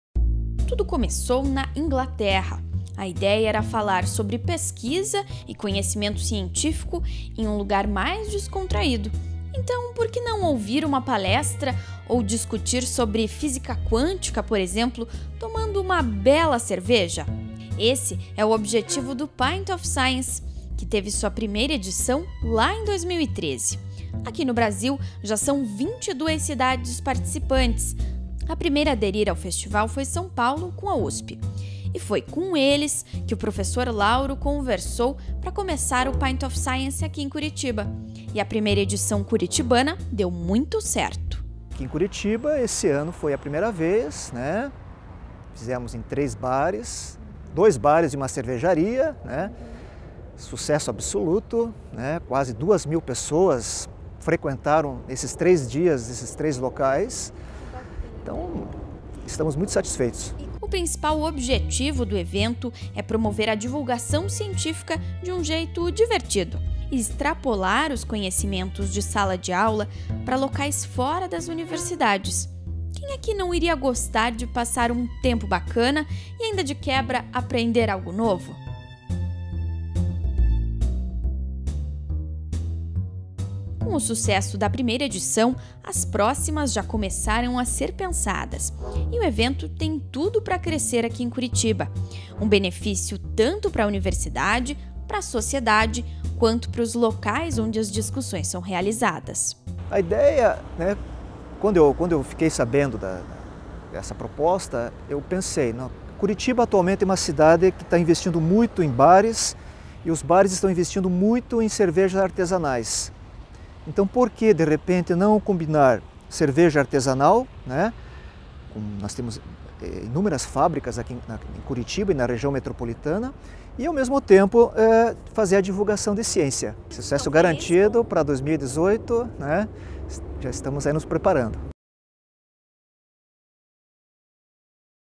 Informa | Festival "Pint Of Science" (29/05/17)
Nos dias 15, 16 e 17 de maio aconteceu o Festival Pint Of Science aqui em Curitiba pela primeira vez!

O evento, organizado pelo Departamento de Física da UFPR, teve como objetivo falar de pesquisa e conhecimento científico em lugares descontraídos.